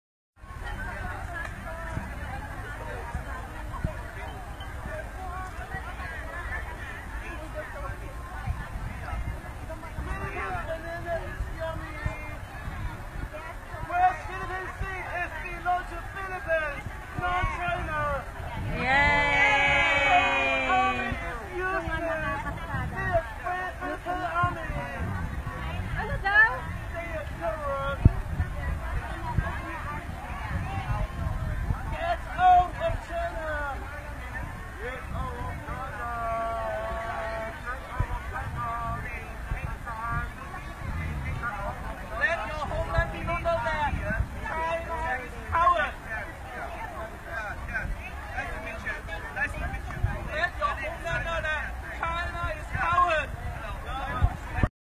Hong Kong citizens love Filipinos and supports Philippines
A group of Hong Kong nationals gathered together to support the Philippines and wants to make peace